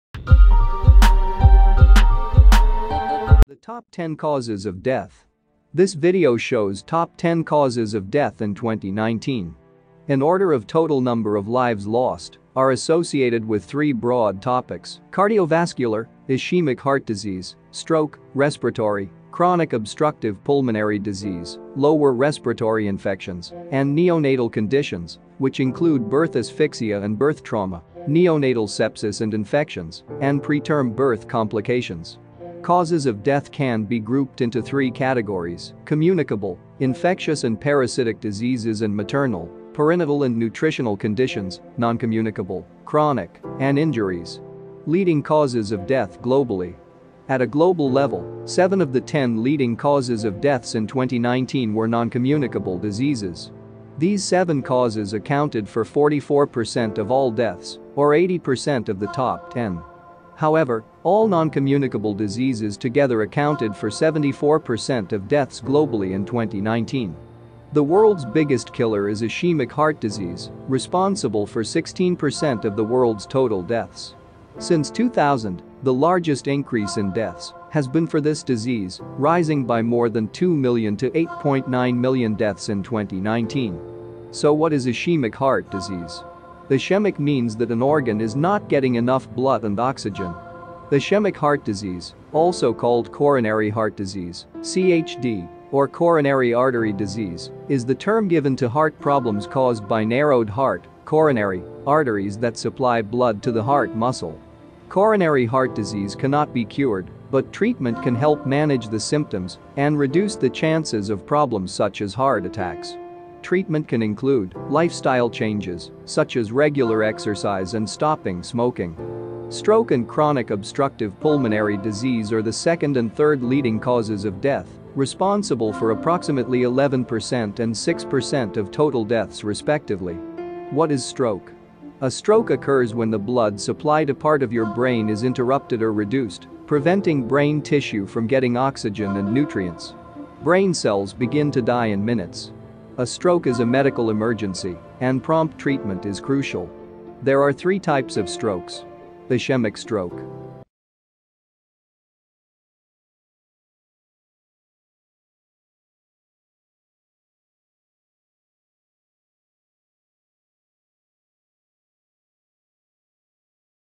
Top 10 causes of death. Reason for death Research 2019/2020 must watch before die
we are working on health sector and we decided to do a research ''causes of death'' and we search our best answer. here why 80% of people die watch the full video this video series continue we will upload our next video as soon as possible every one should watch before die because .
this video is made for education &
our next video series will be ''how to get a better & healthier life''
mind this food is medicine
control your food and heave a healthier life